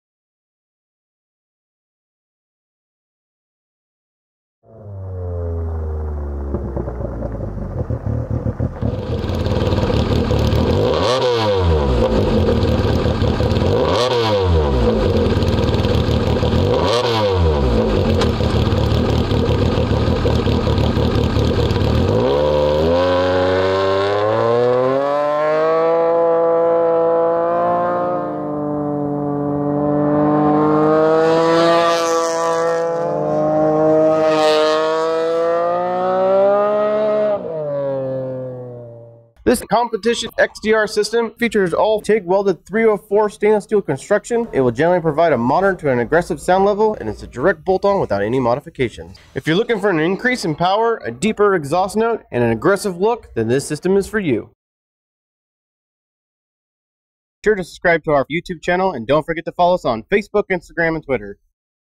2018-19 Arctic Cat Wildcat XX - XDR Off-Road Competition Single Exhaust System 7103
2018-19 Arctic Cat/Textron Wildcat XX - XDR Competition Single Exhaust System 7103.

Questions? Need an exhaust or intake recommendation for your vehicle?

In this video you will hear and see the awesome power of the new XDR Competition Single Exhaust System for the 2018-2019 Textron ArcticCat WildcatXX 1000 side by sides. The new XDROffRoad Competition single exhaust system utilizes full 304S stainless steel construction, bolts up to the factory flange and is firmly held in place using the factory mounts and springs. Features include a large 2.50-inch inlet flange that mates up perfectly to the factory flange and is secured by re-using the factory exhaust springs. The high-flow compact round XDR competition race muffler exits through a large 2.50-inch mandrel-bent tailpipe. The cheap plastic factory heatshield is replaced with a black powder-coated 304S stainless heatshield sporting a laser-cut XDR logo and improved venting design that not only looks good, provides better airflow and heat dissipation, but also makes the statement that you're serious about performance.

• Quick and Easy Installation Using Factory Mounts
• TIG-welded 304S Stainless Steel Construction
• High Performance XDR Competition Muffler
• Deep Powerful Sound